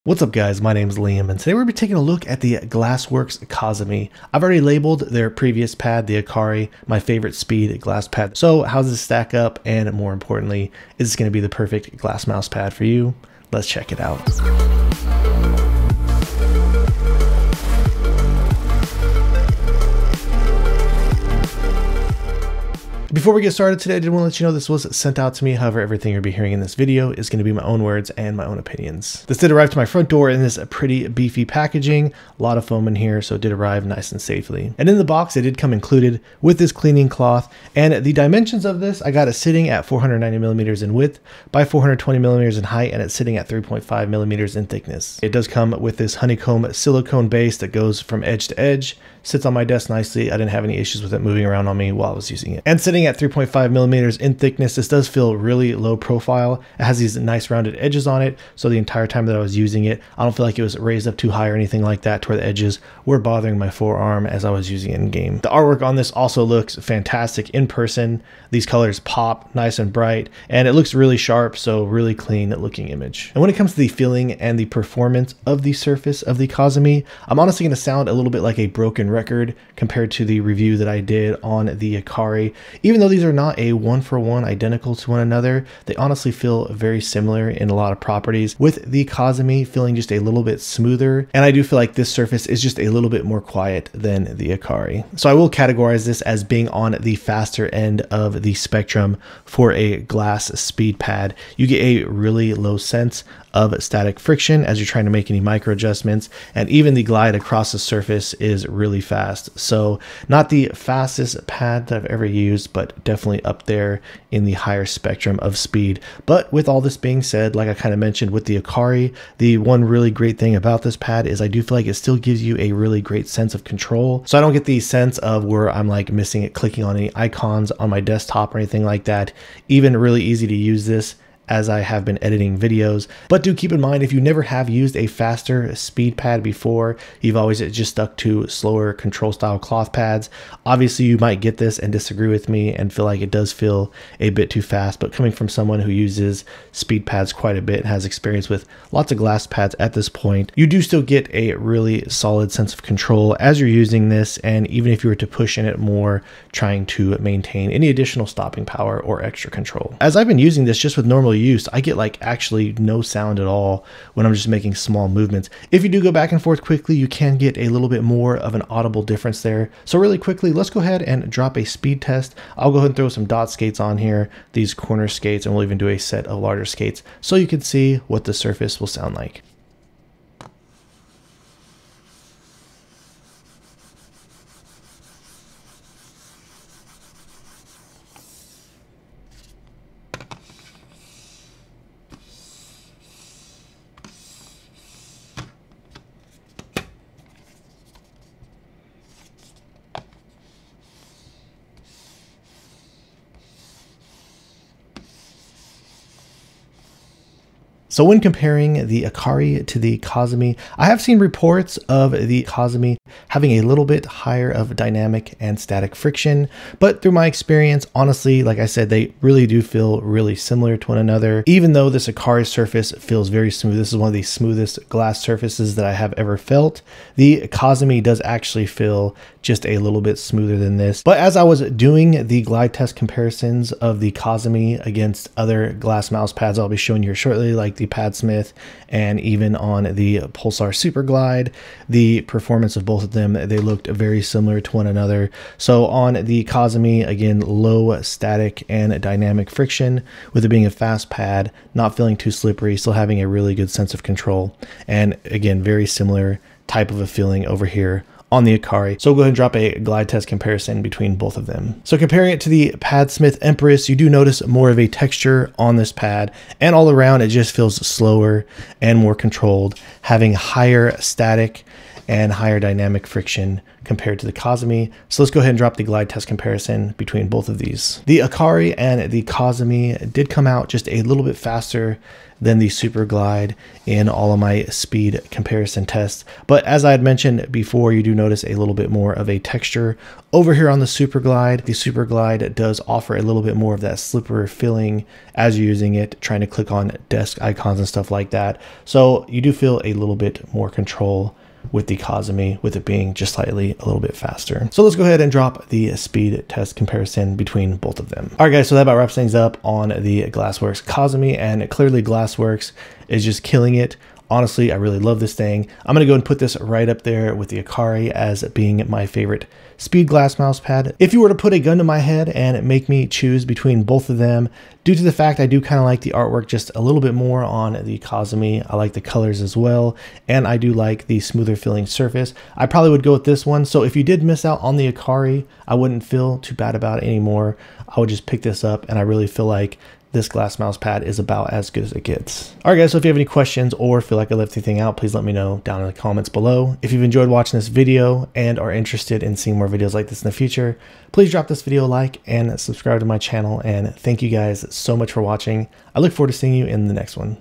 Smoothest Glass Pad So Far (GLSSWRKS Kazemi)
00:00 Intro
00:34 Size
00:45 Base
00:52 Edges
01:13 Surface
01:34 Performance
02:59 Sound test
03:27 vs Akari
04:21 vs Padsmith Empress
04:37 vs Pulsar Superglide
05:08 Final thoughts
Save $ with code: AimAdapt

Hand Size = 18.3cm x 10.5cm